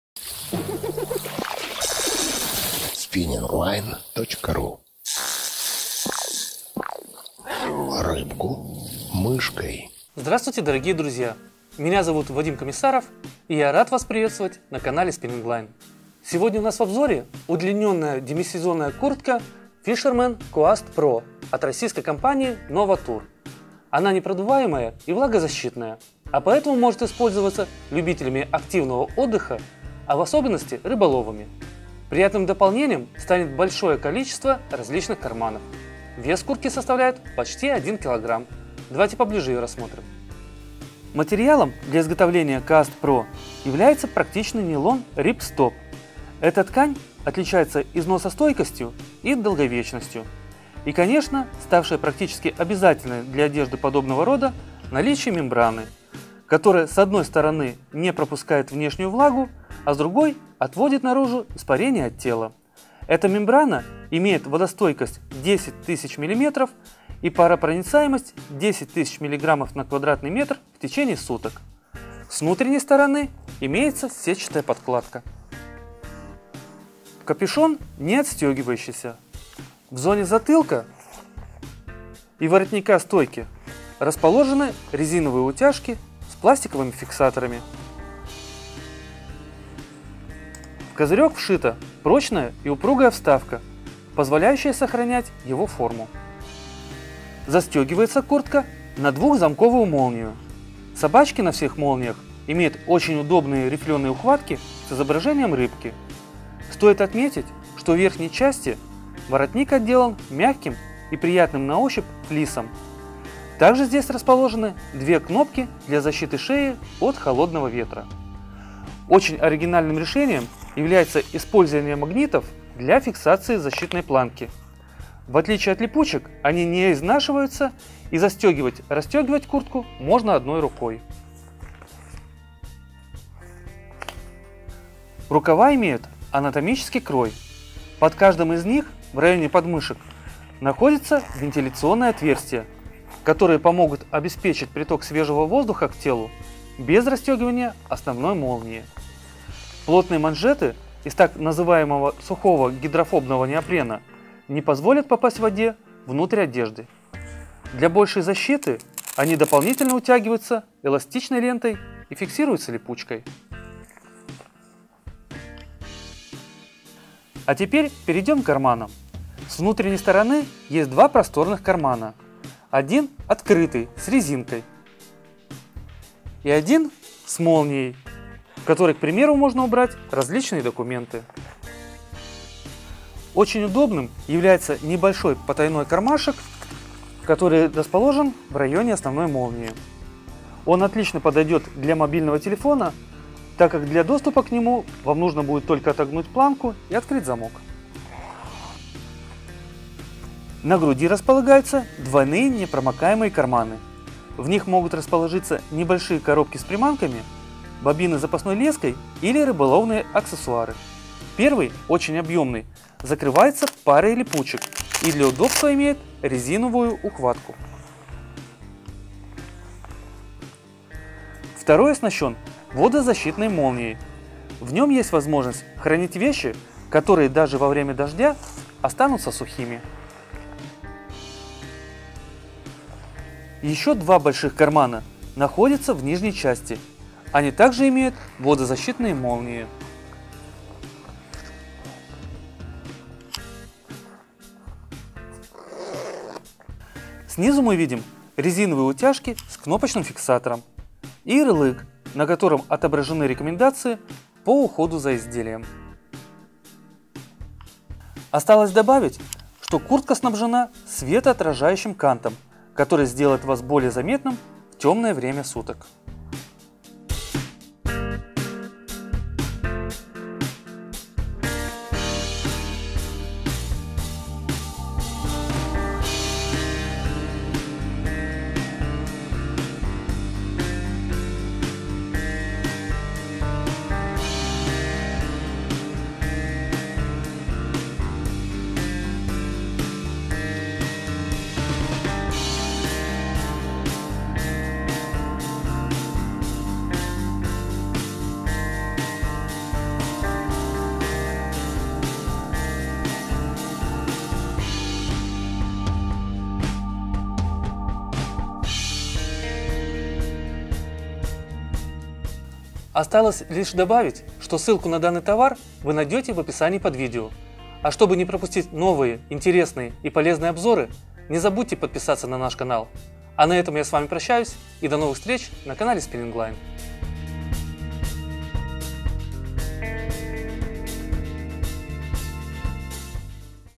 Куртка Fisherman Nova Tour Коаст Pro
Купить куртку Fisherman Nova Tour Коаст Pro Fisherman Nova Tour Коаст Pro
При изготовлении удлиненной куртки для любителей береговой ловли Коаст Pro использована мембранная ткань Nylon Ripstop. Основная двухзамковая молния оснащена внутренней планкой для защиты от возможности продувания. В конструкции куртки предусмотрен анатомически скроенный регулируемый вшивной капюшон, который легко убирается в воротник стойку. На куртке Коаст Pro имеется 2 прорезных кармана на молниях в верхней области и 2 кармана, закрытых цельноскроенными клапанами, внизу, плюс удобный внутренний карман на утяжке-резинке. На внутренних областях рукавов имеются вентиляционные отверстия с молниями. Благодаря скроенным с учетом возможных движений рукавам и отменной посадке по фигуре в куртках модели Коаст Pro удобно рыбачить различными способами.